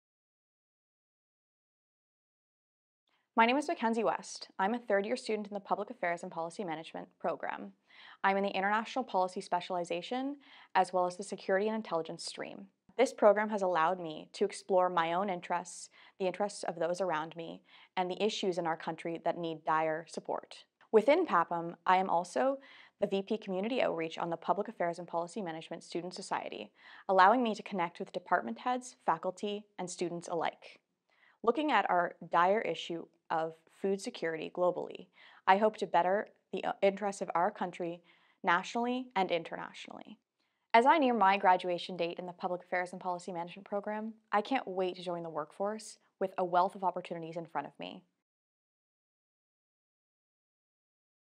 Mackenzie West, 2021-2022 FPA Dean's Honour List
Mackenzie West, BPAPM
Learn how you specifically learn best; everyone is different so start practicing habits which help you in your day-to-day life and scheduling your study, class and personal time.